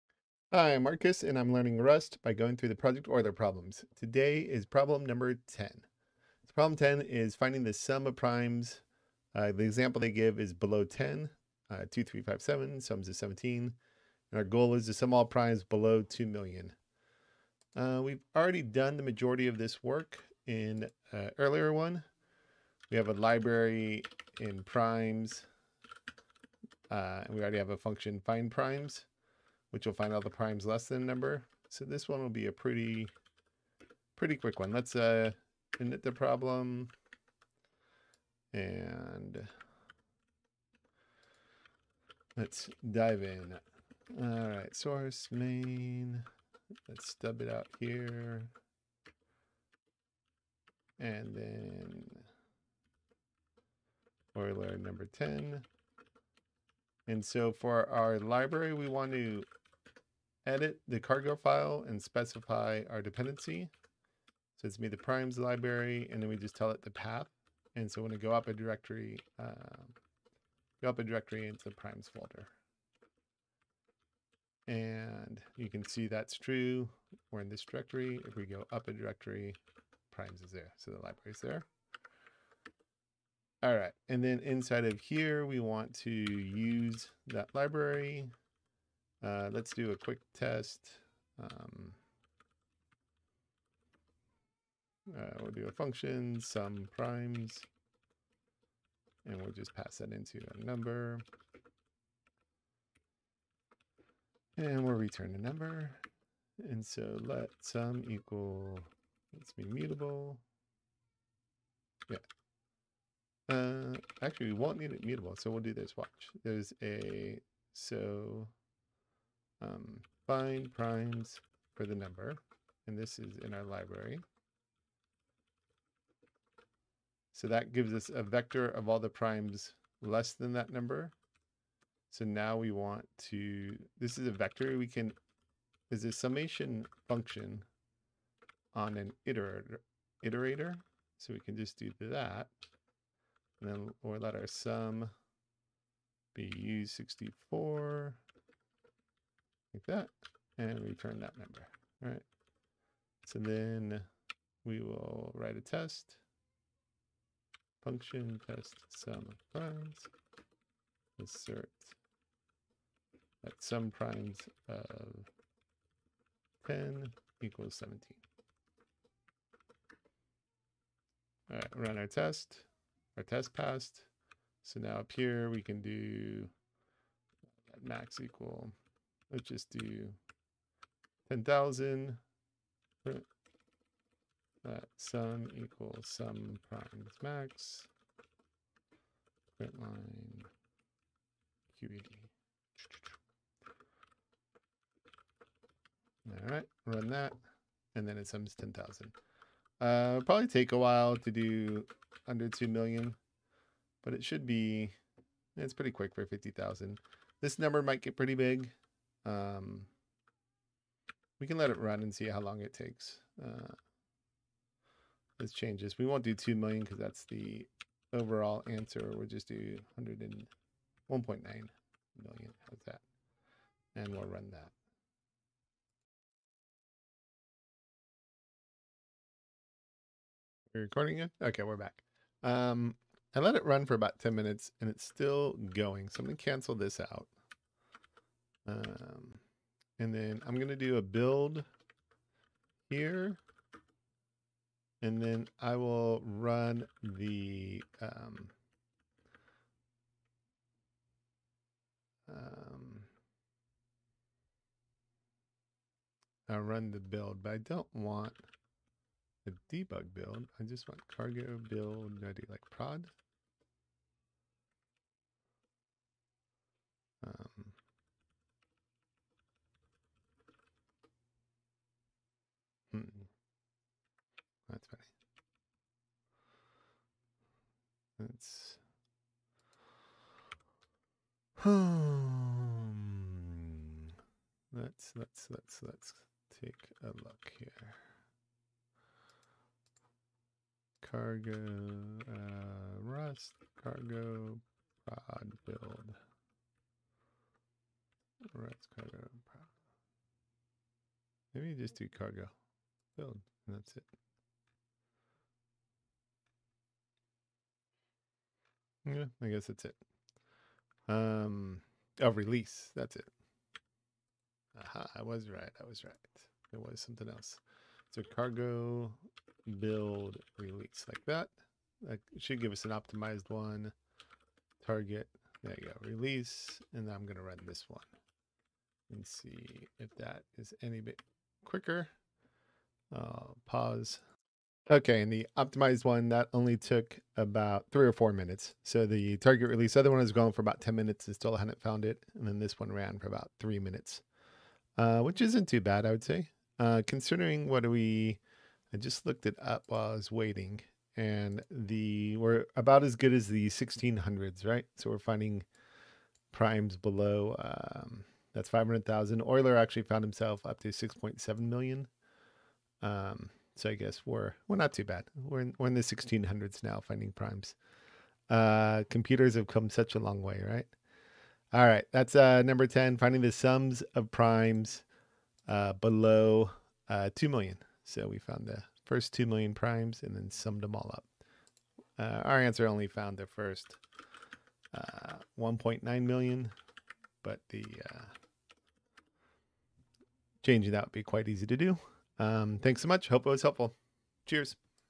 Euler #10 in Rust - Summation of primes
A screencast series learning Rust by solving Project Euler problems.